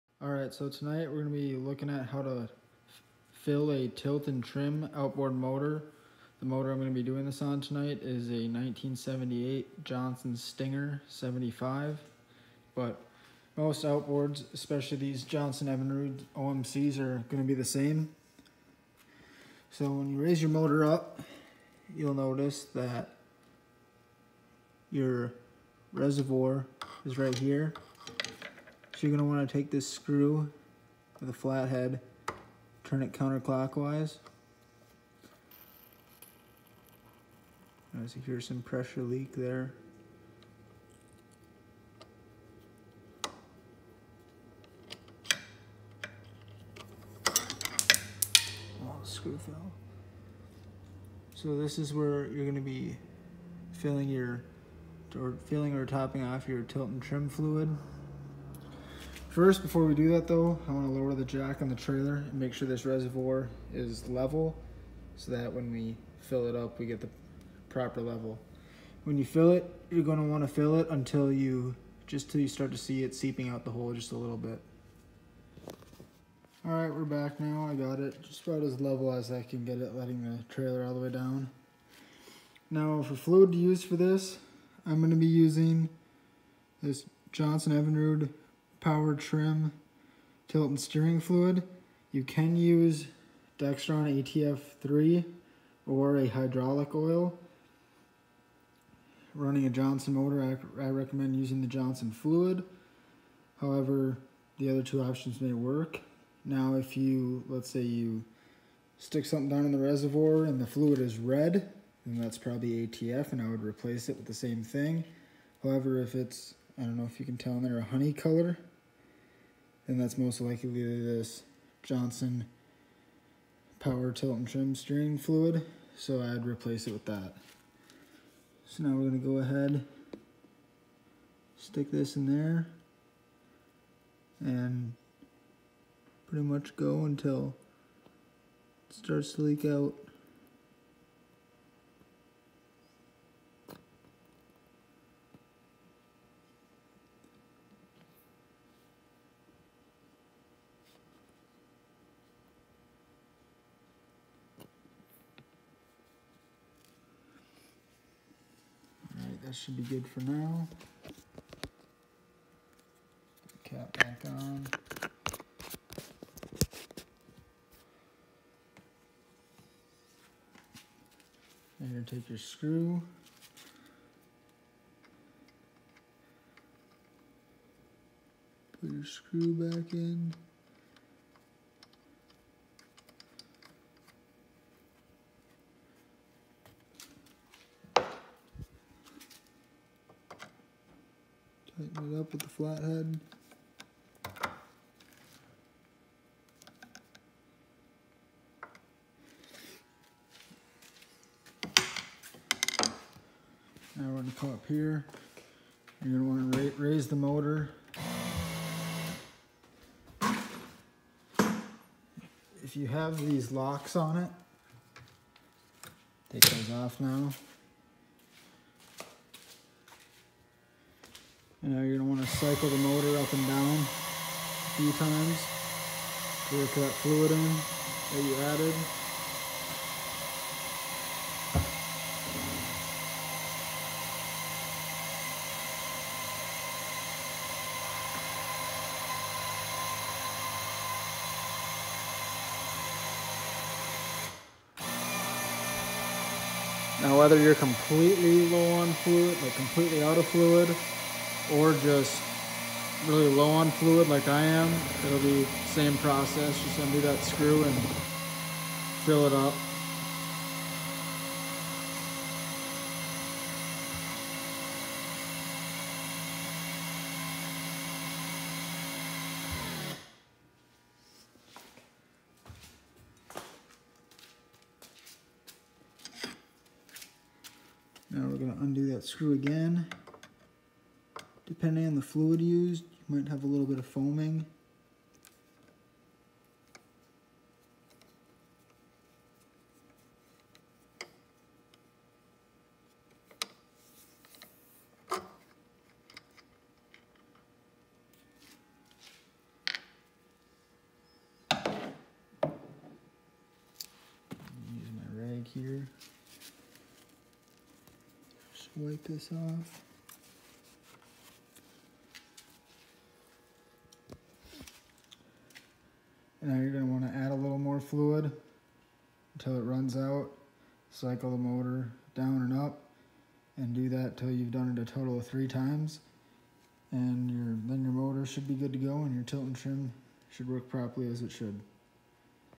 Outboard tilt/trim fluid fill
Filling the tilt trim fluid reservoir on a 1978 Johnson stinger